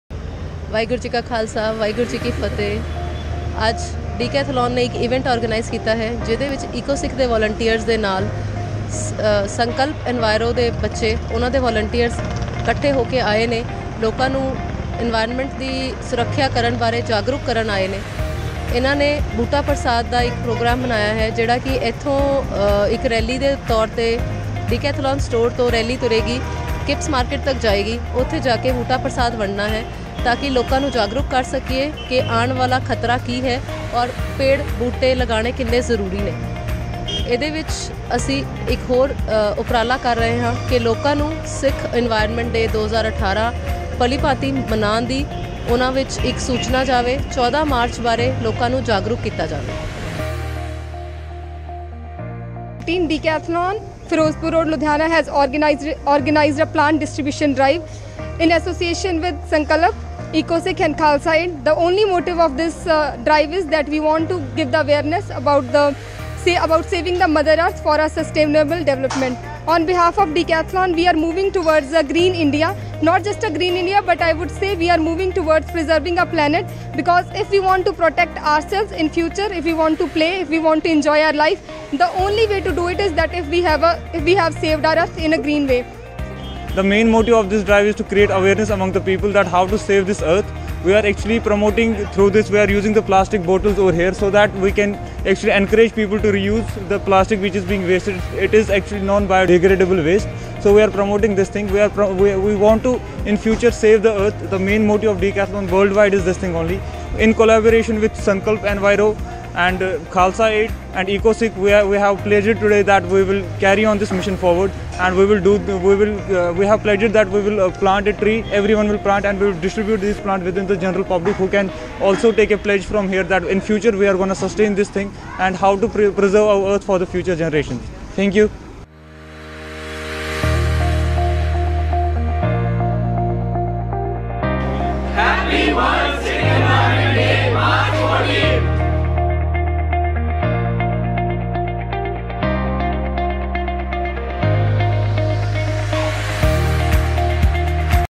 An environmental drive by Decathlon,Ludhiana with EcoSikh and Sankalp.
EcoSikh Ludhiana team along with Sankalp Enviro was invited by Decathlon Ludhiana for an environment rally on jan 26,2018. EcoSikh team inspired the public to celebrate World Sikh Environment day on March 14 and shared there ideas, about it.. Register your organization at our